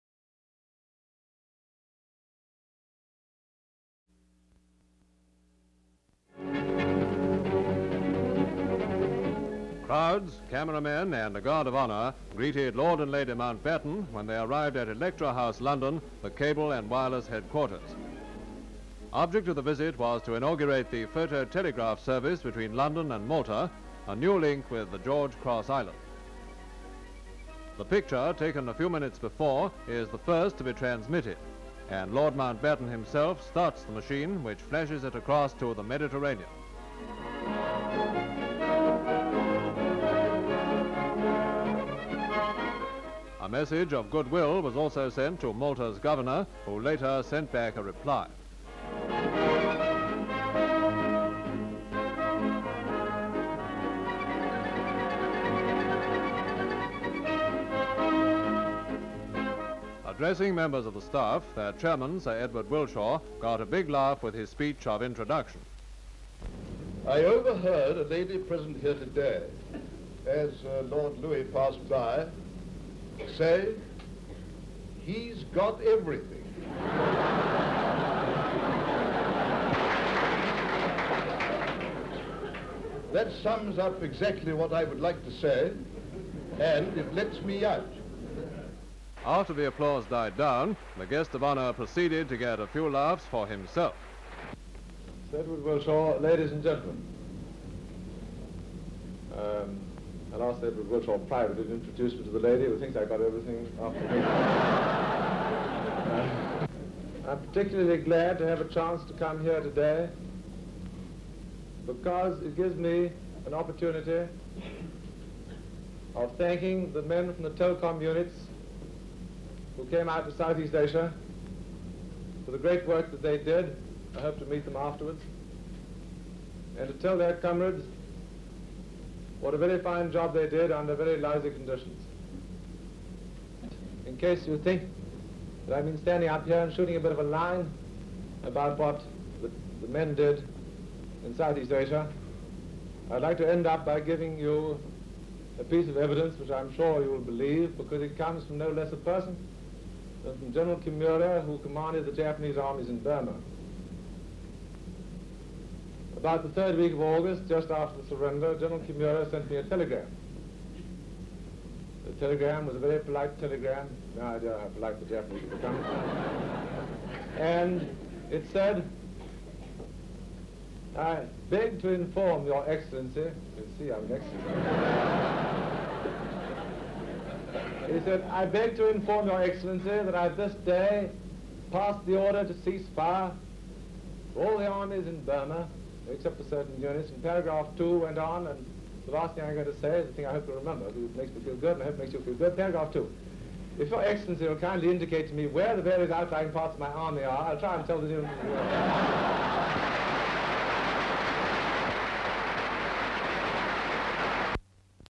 Lord Louis Mountbatten arrives at Electra House, London to inaugurate a telecommu...HD Stock Footage
Lord Louis Mountbatten arrives at Electra House, London to inaugurate a telecommunication service.

Lord Louis Mountbatten inaugurates telecommunication service between Malta and England from Electra House, London. Car of Lord Mountbatten arrives. People stand besides road with umbrellas.Mountbatten takes off his over coat, his wife stands besides him. They pose for photographers with chairman of London Telecommunication services. Lord Louis rolls picture to machine and presses a button. Governor of Malta gets the message of goodwill signed by Lord Mountbatten. Men work at Telecommunication office. Chairman, lady and Lord Mountbatten address workers of Electra house. Chairman speaks. Lord Louis comes and speaks. He thanks and appraises workers of telecom unit. He speaks about the telegram he got from the Commander of Forces in Japan. People present in the hall laugh at the humorous incident. Location: London England United Kingdom. Date:  1943.

57,000+ broadcast-quality historic clips for immediate download.